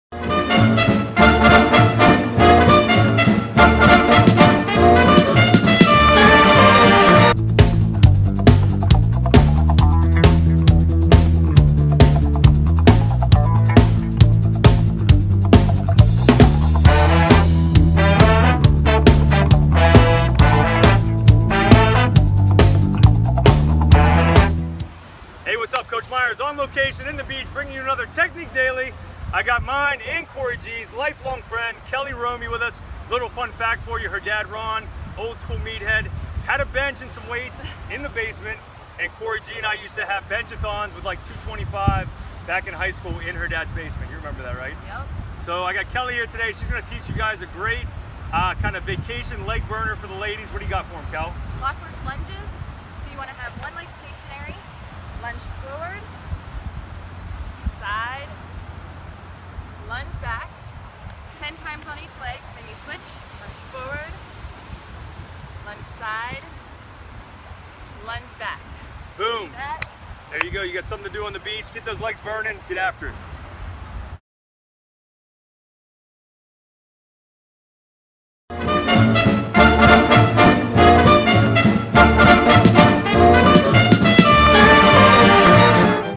Clockwork Lunges | Coach Myers & Kelly Terwilliger
On this episode of Technique Daily, Coach Dustin Myers is joined by Kelly Terwilliger who is showing a great lunge variation! Give this one a try!